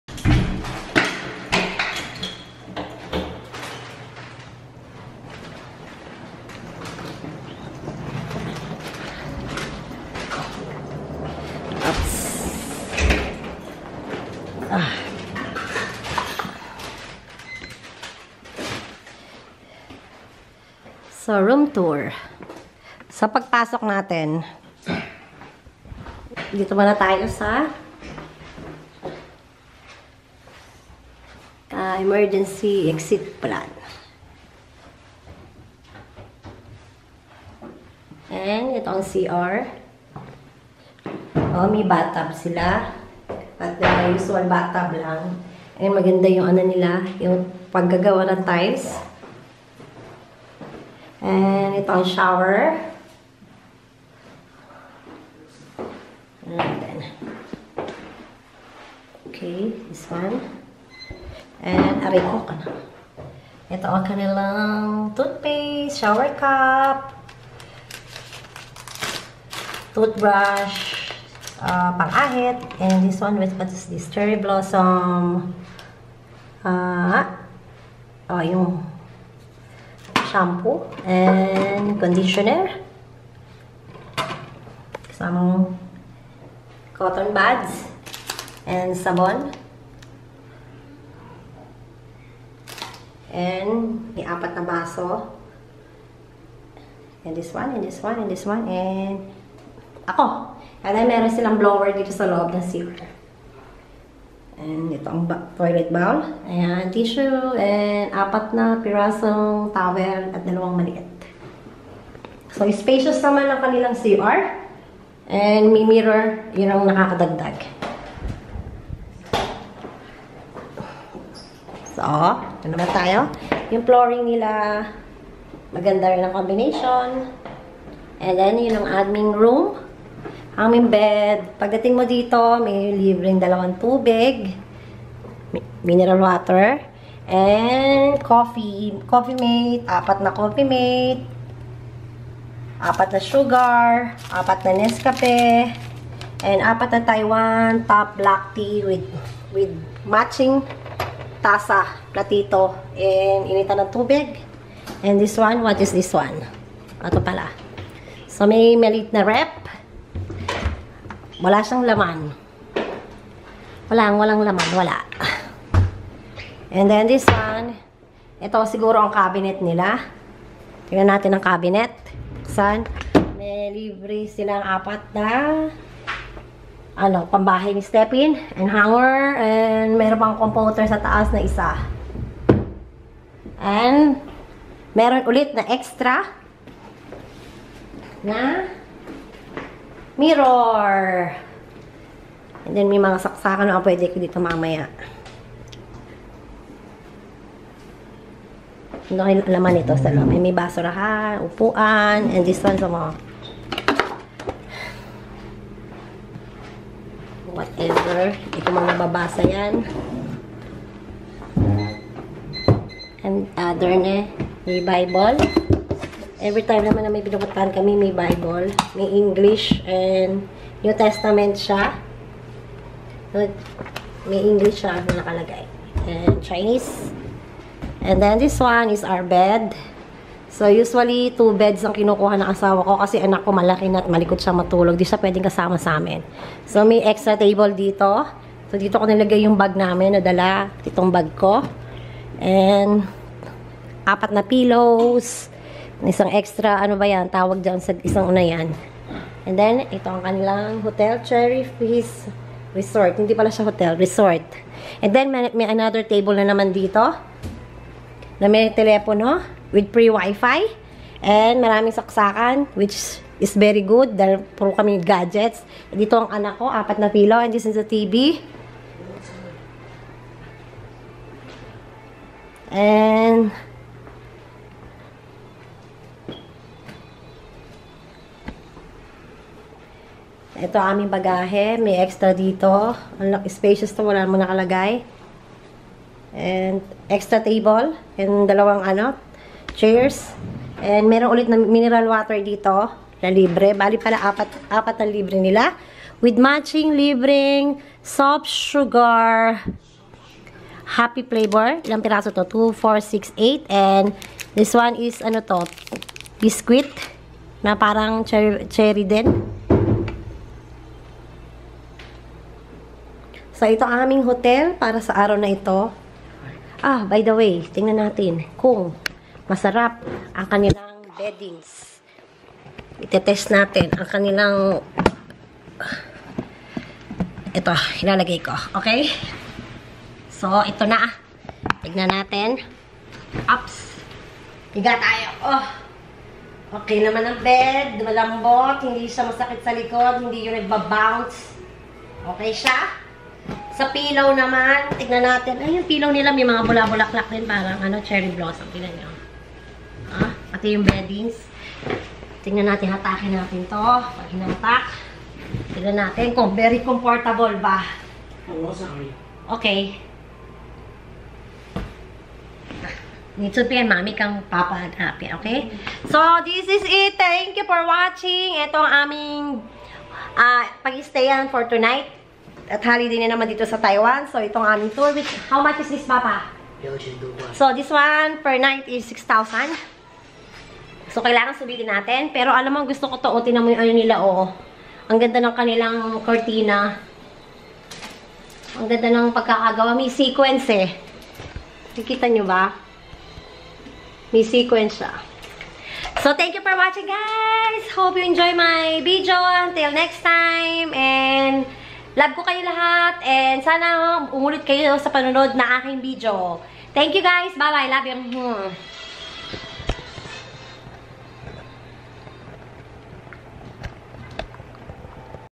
cherryfeastresort Cherry Feast Resort (Room Tour) Nantou Taiwan
Our Hotel is a few minutes away from the famous Sun Moon  lake.Our Room are very spacious,comfy and calm.Breakfast is free for 3 persons.They don't offer Dinner but It doesn't matter because night market is a few steps away  from the Resort too.Restaurant  is too many to choose nearby  plus some stuff you can buy from Sun Moon Lake